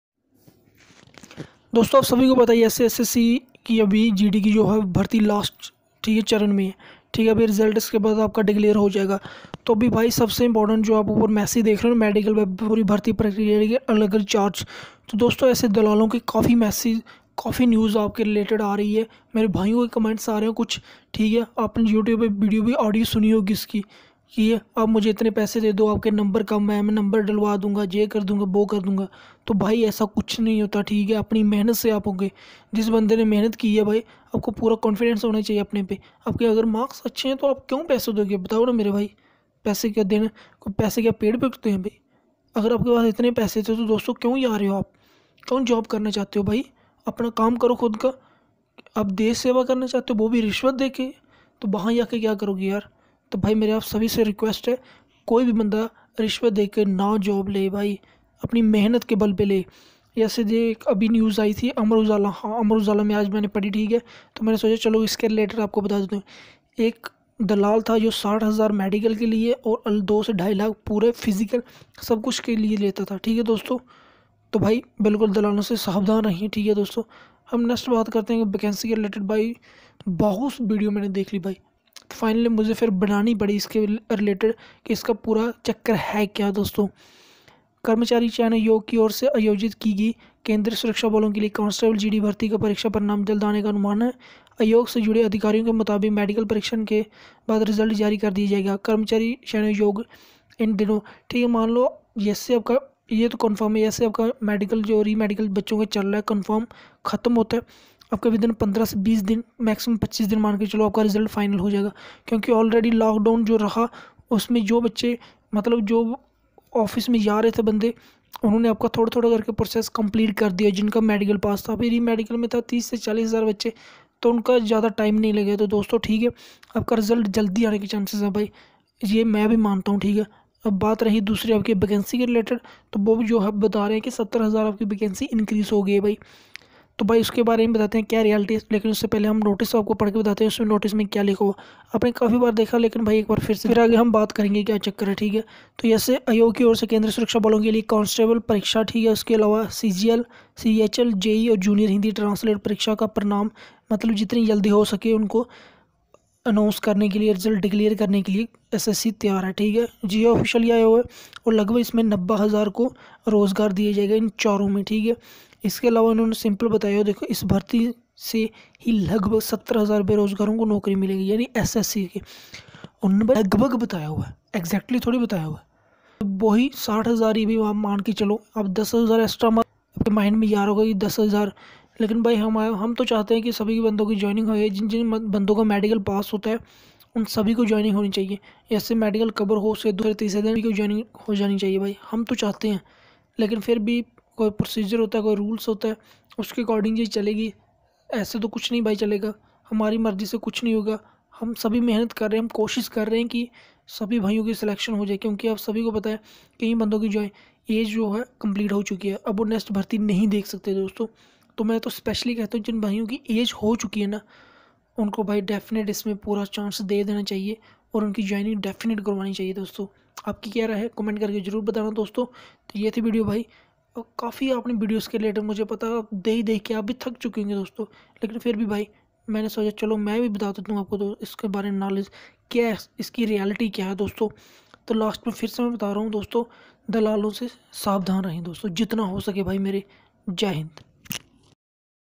दलालों से साबधान ! |SSC GD VACANCY INCREASE GOOD NEWS 70000
all candidate of ssc gd 2018 beware of tout
selection is made pure basic so please don't give any bribe to some one
 this is my personal view I don't have any claim to do any thing wrong